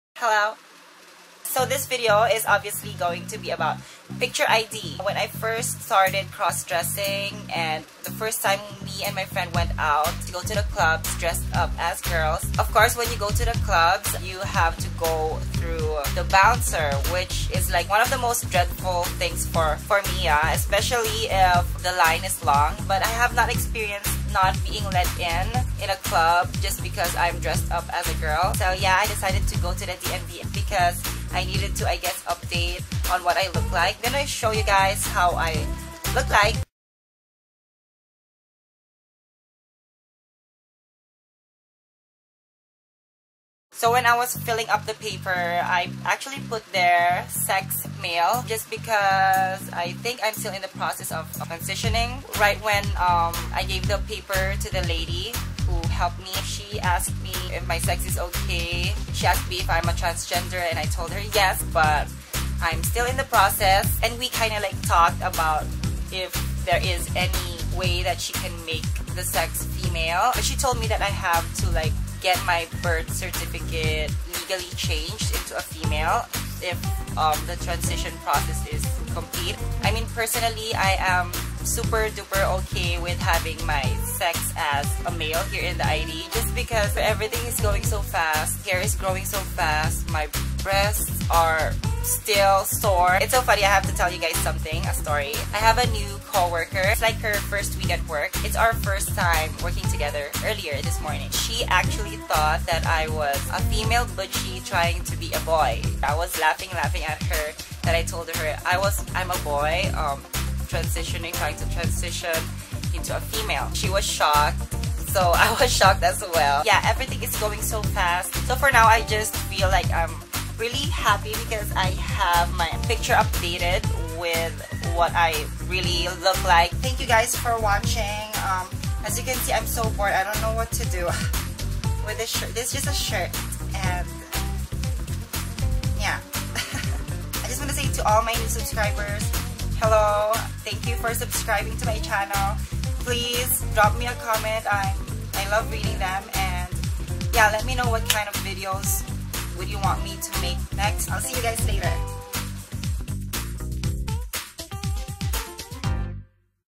The Picture ID
I've been wanting to update my Picture ID for the longest time and finally did it! Went to the DMV armed with a full makeup, a cute outfit and a smile to brighten up the entire boring DMV room.